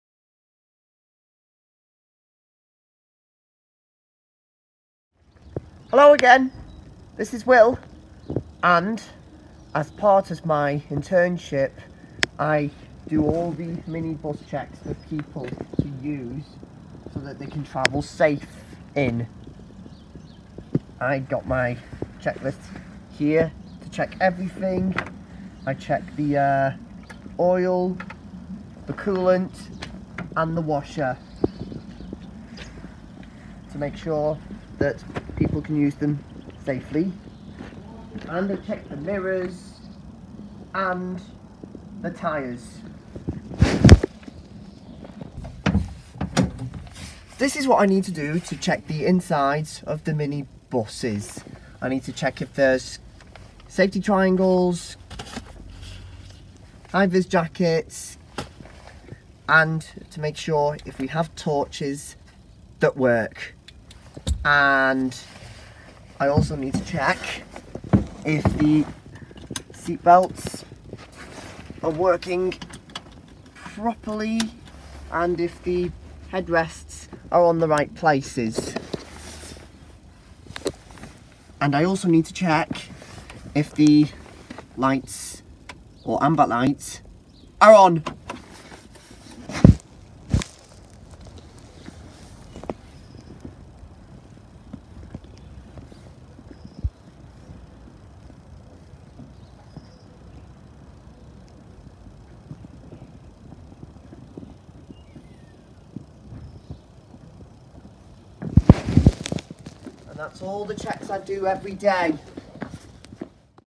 Weekly Minibus Checks
In our final video this week in support of National Careers Week we see Will, our supported intern, carrying out important weekly minibus checks!